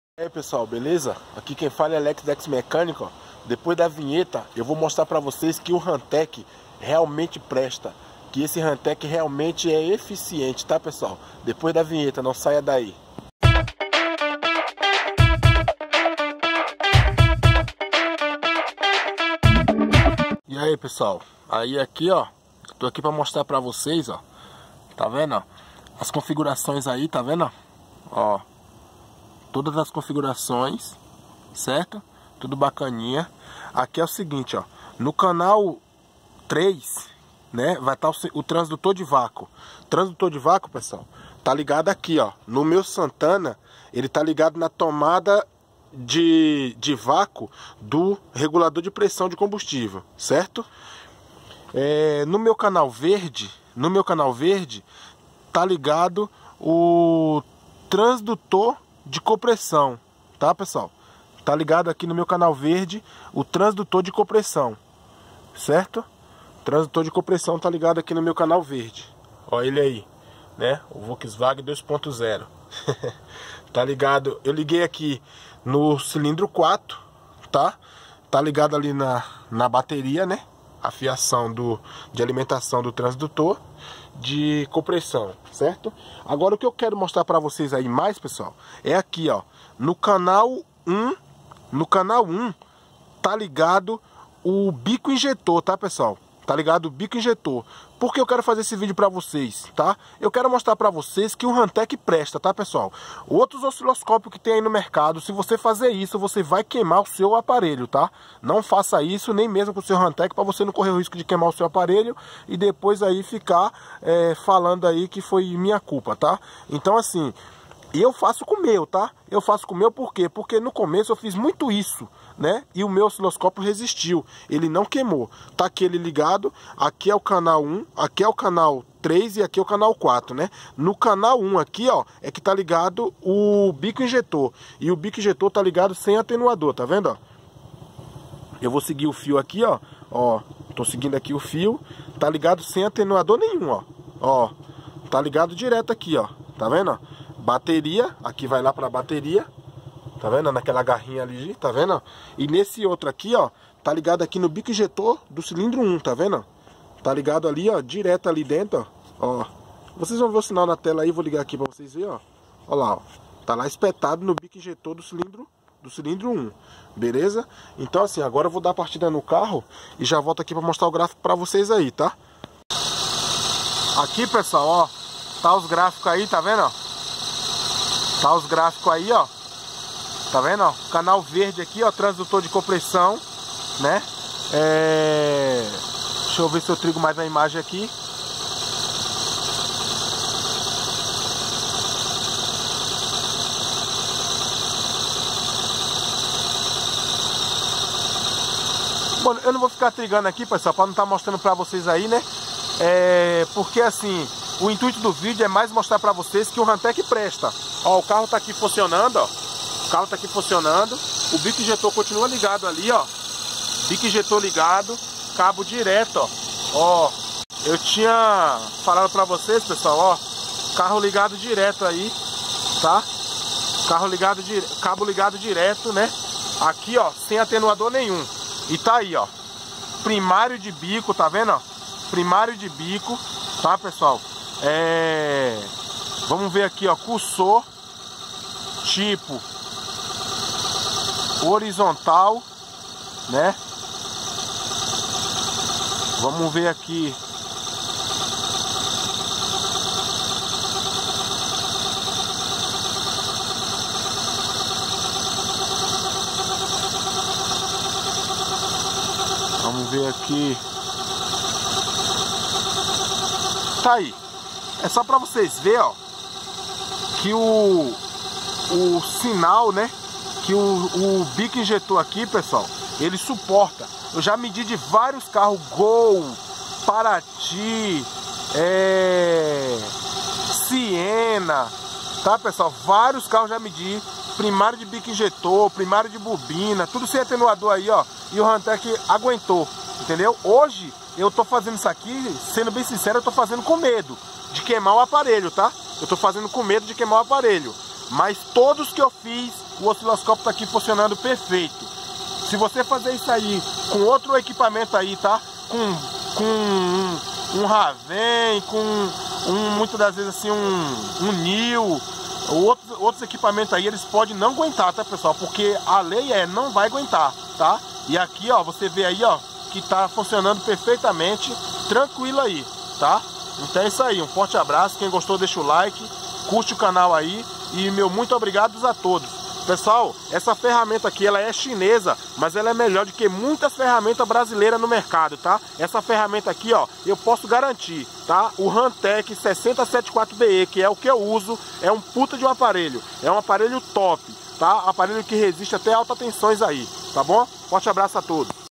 Veja porque você deve comprar um Hantek 6074be
Neste vídeo eu mostro eu usando o osciloscópio pegando sinal de bico sem atenuador...e o aparelho não queima...